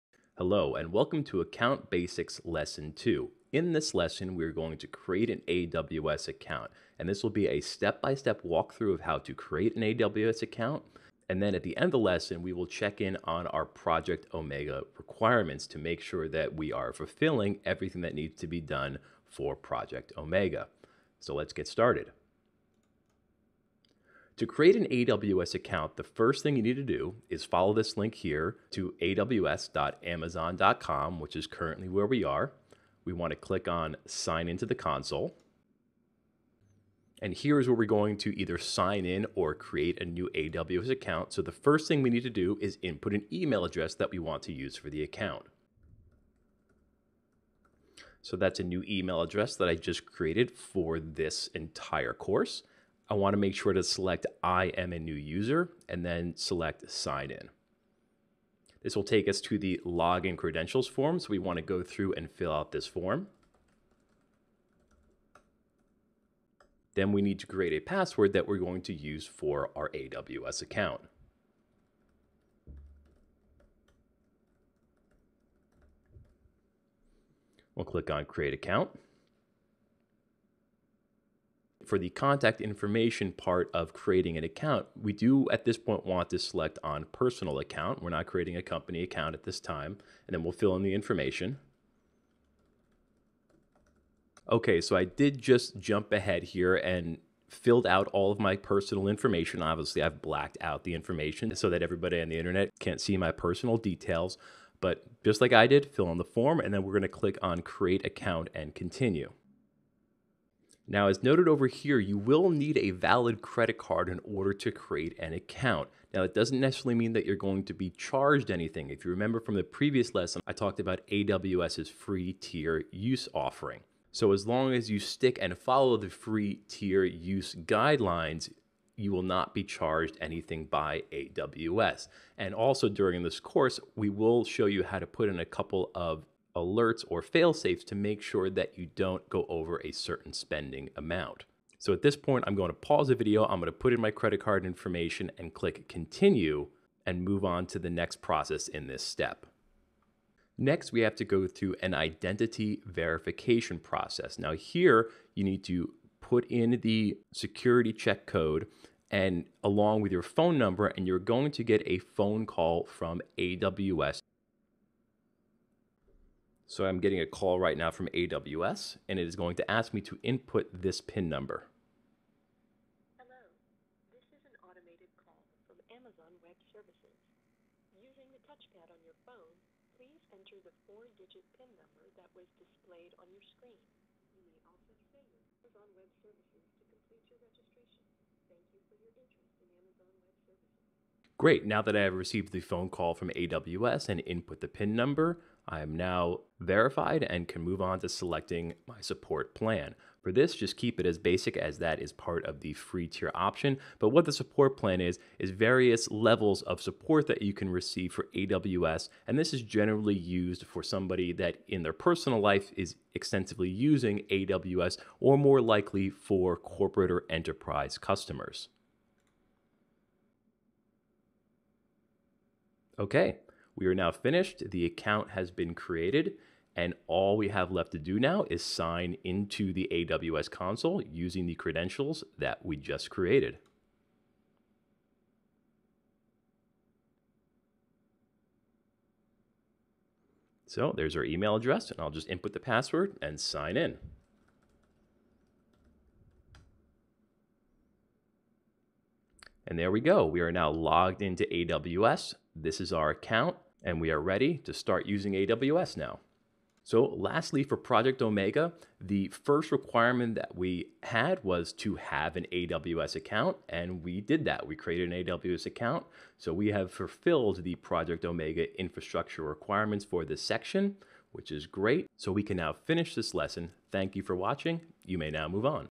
AWS Essentials: Create an AWS Account
Ever wondered what steps are involved in creating an AWS account?